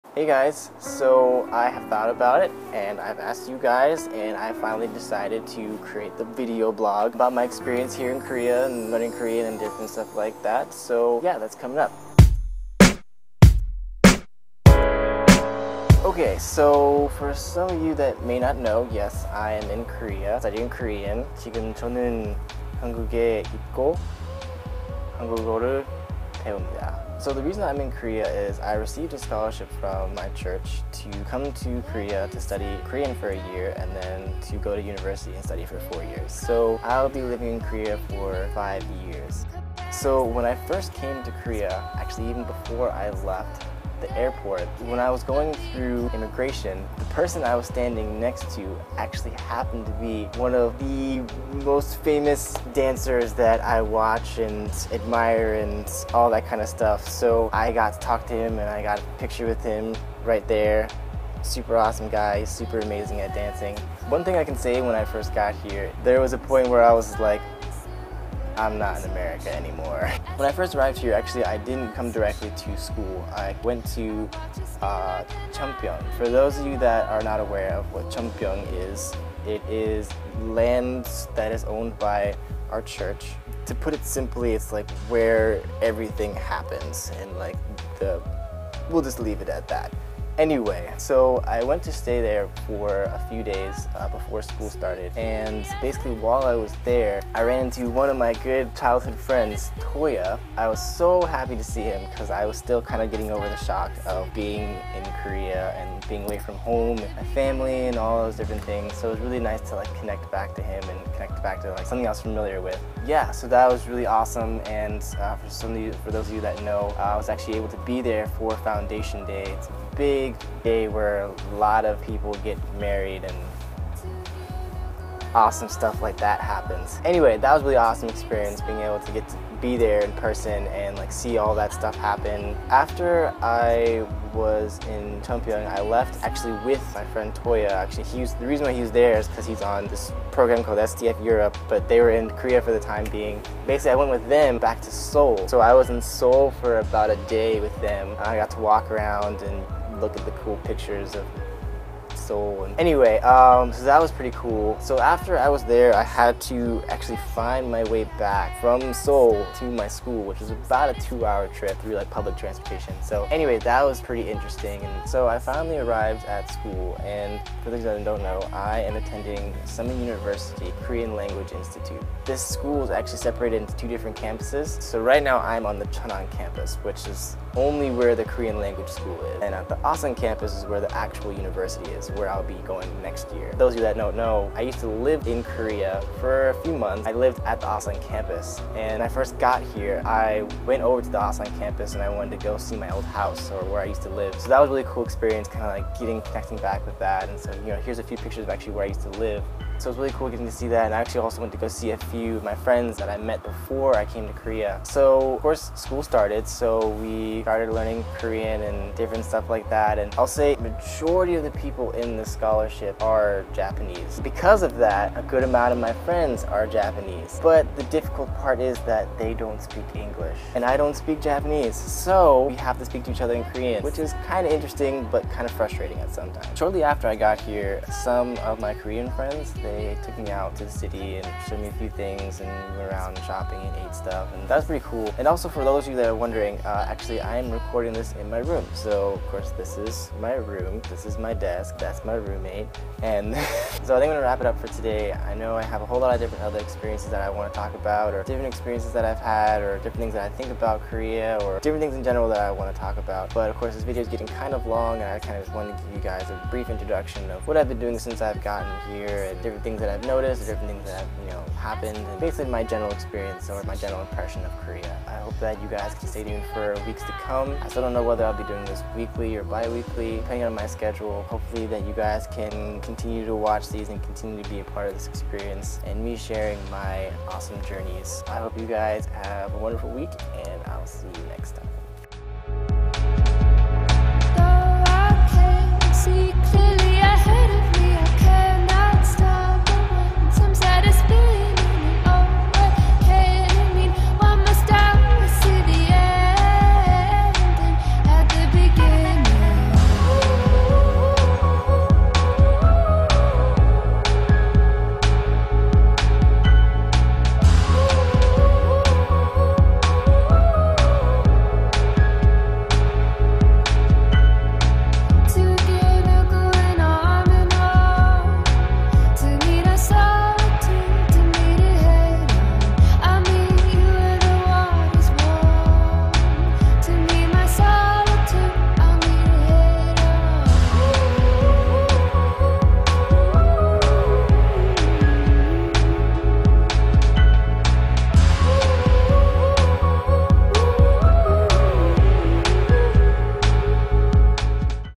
Coming to Korea | Imani P.M.
This is the first episode of my adventures in Korea. This will briefly go over the things I have done since I arrived and my experience. Hope you enjoy!